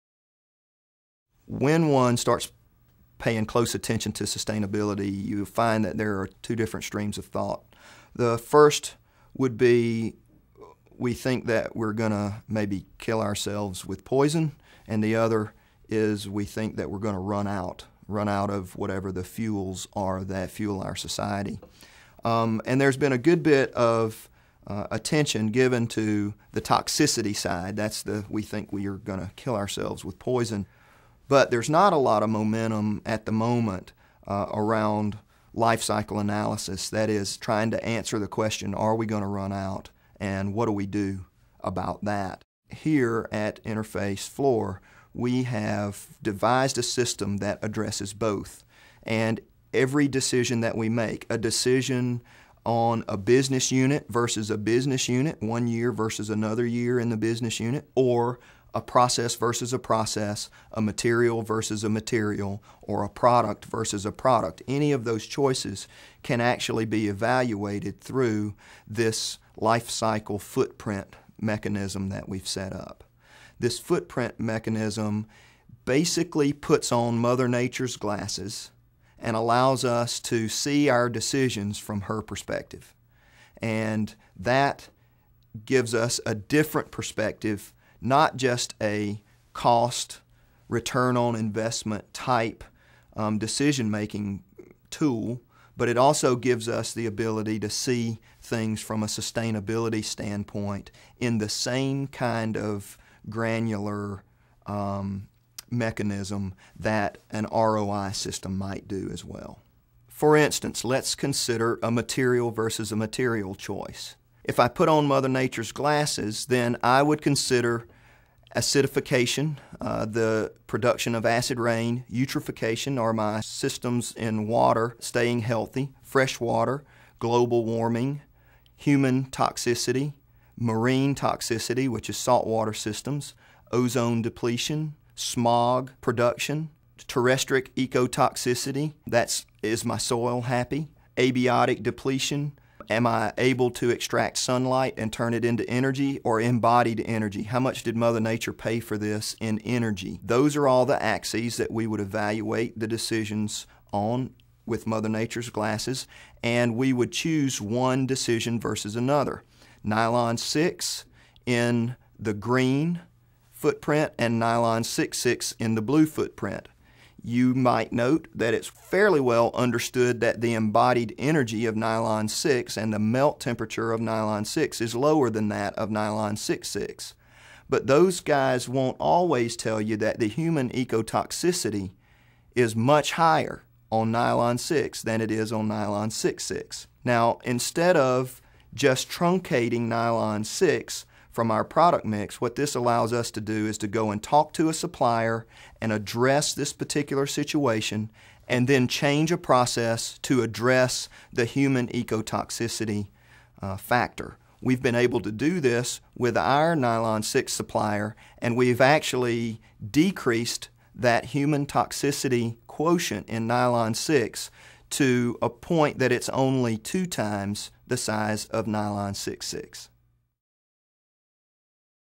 InterfaceFLOR Life Cycle Analysis by John Bradford
Life Cycle Analysis in InterfaceFLOR's decision making.